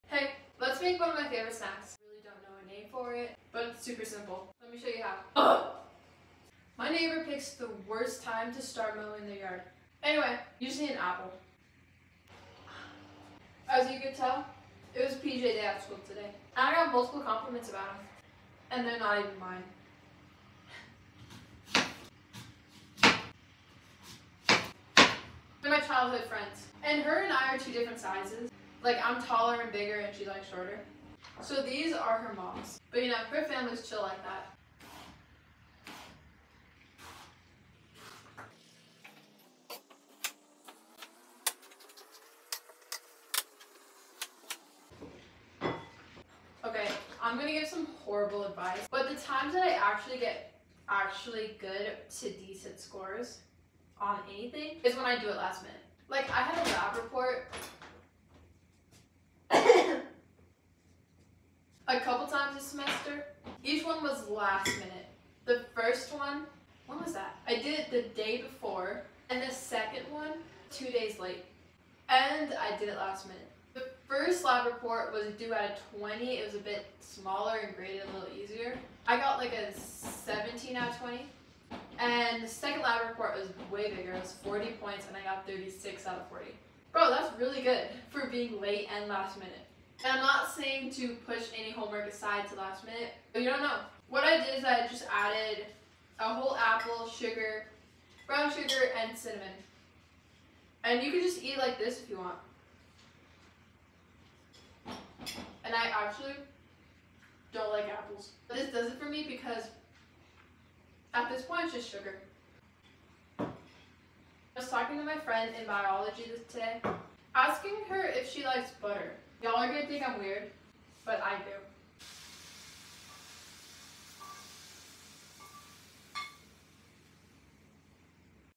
Apple BITES?
Has there ever been a time where you did a large project for school last minute, but it STILL got a good grade?!?!?! Lemme know!!

Honesty y'all, there aren't any serious measurements when it comes to these apple snacks. Just add whatever your HEART desires. that's what I did 😂.
(Apples, brown sugar, and cinnamon. Go play with the recipe!!)